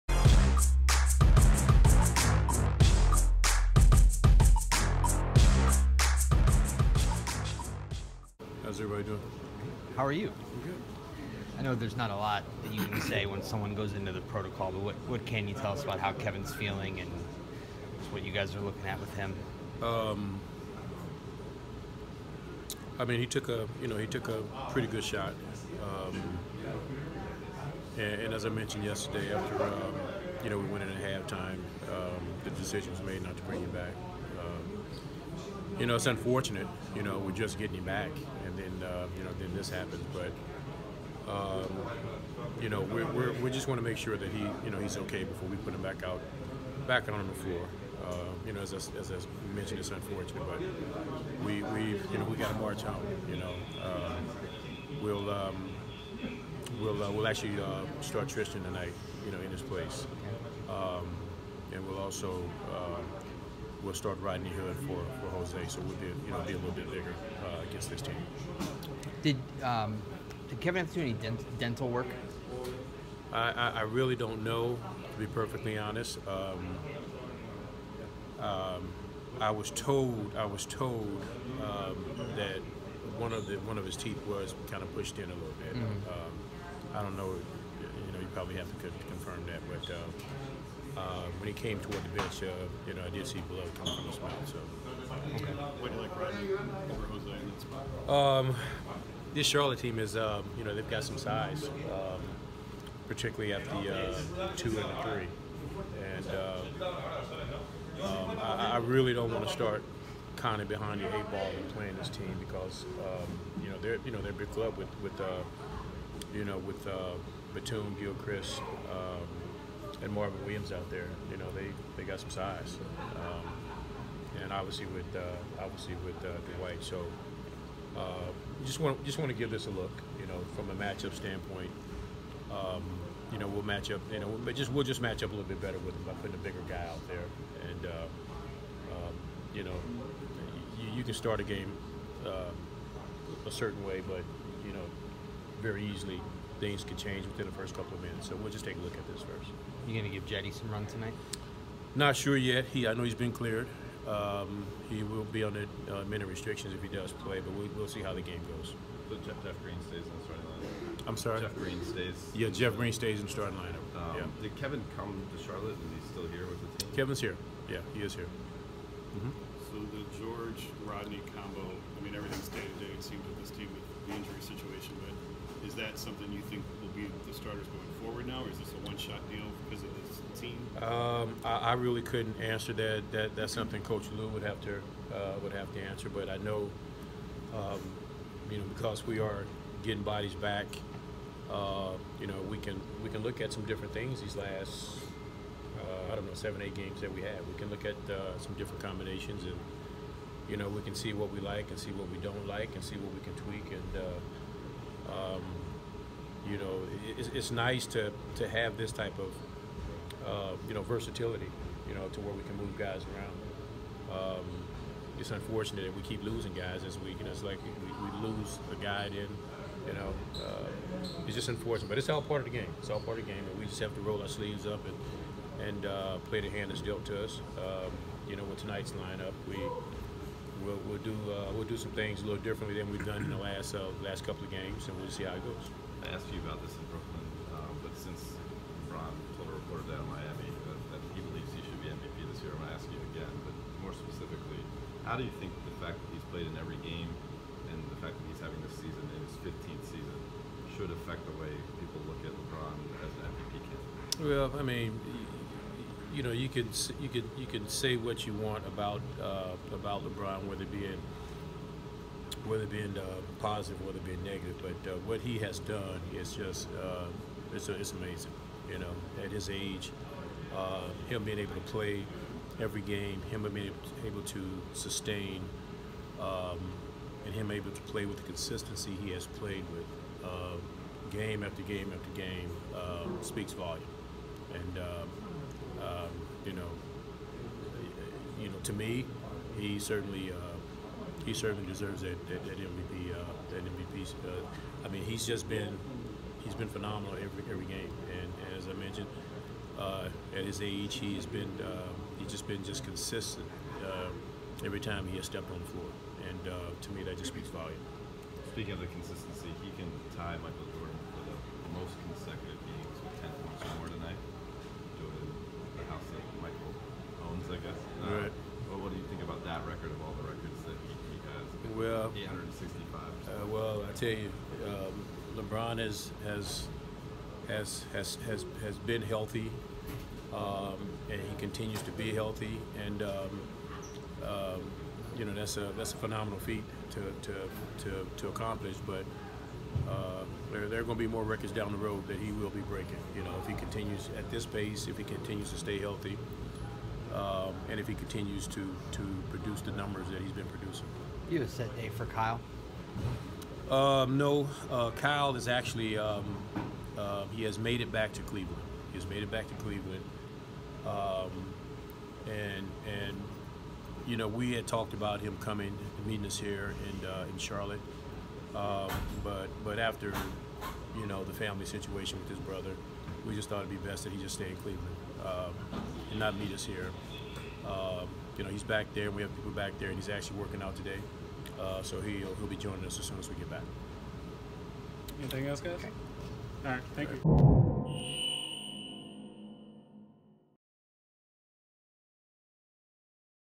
Larry Drew discusses losing Kevin Love again and Kyle Korver's imminent return
Cavaliers acting head coach Larry Drew speaks before a game against the Charlotte Hornets on March 28, 2018.